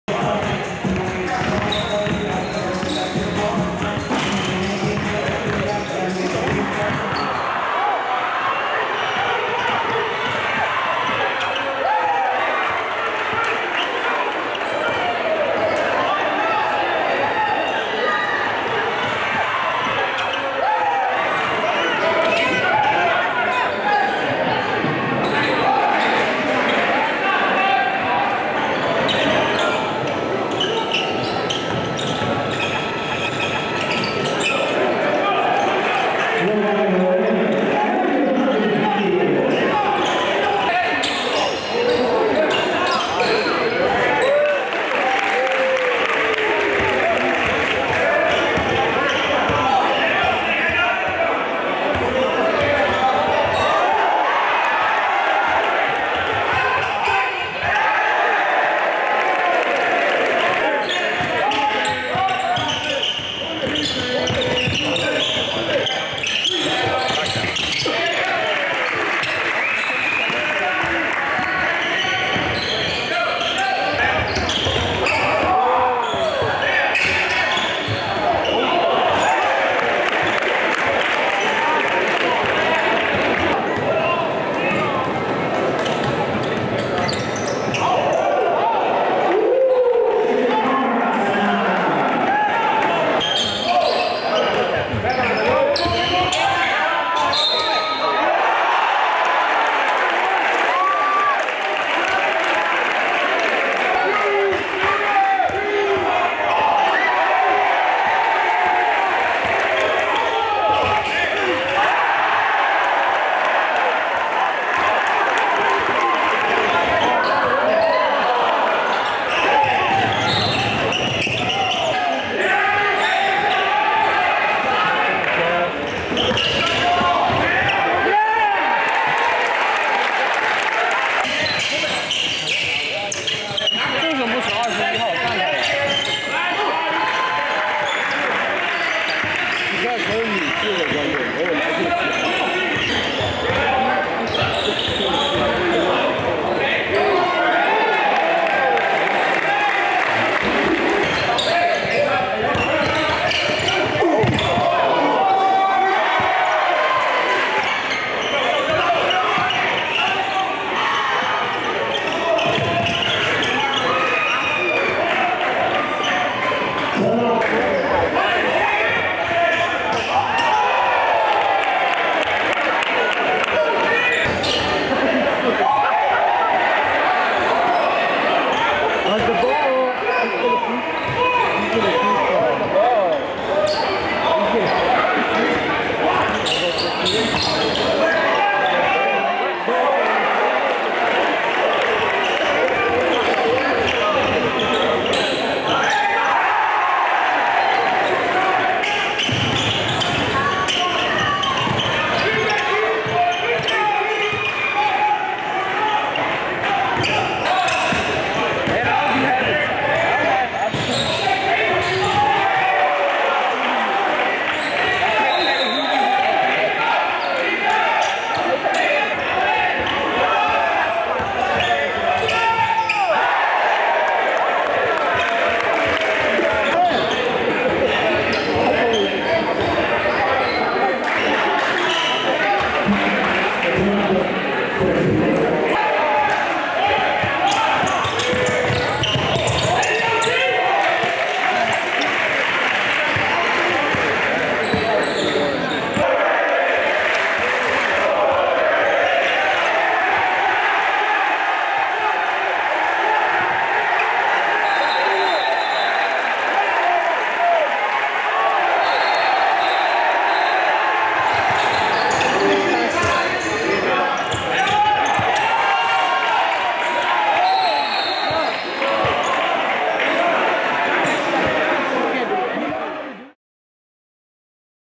Chikara Tanaka Highlights With IMG Academy! Elite PG Out Of Japan!
2020 Courtside Minneapolis Camp - June 6th!